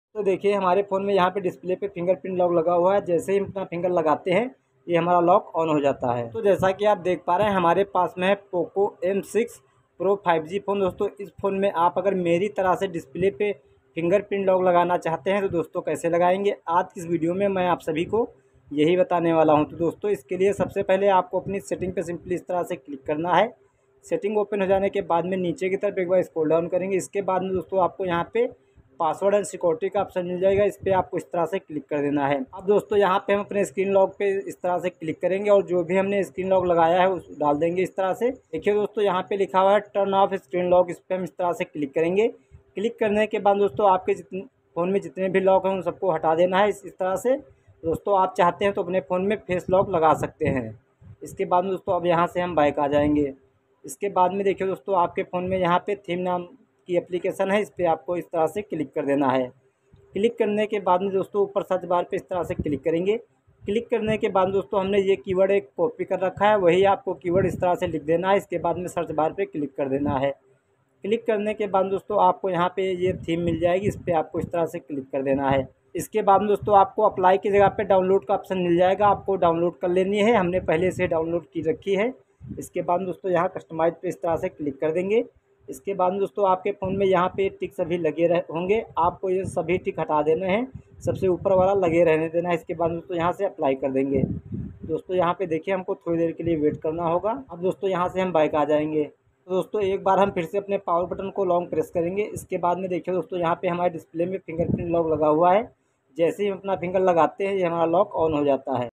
Poco m6 pro 5g display fingerprint setting /poco m6 pro me in display fingerprint lock Kaise lagaye
Hello doston aaj ki is video mein humne aapko bataya Poco m6 pro 5g mobile me fingerprint lock Kaise lagaya jata hai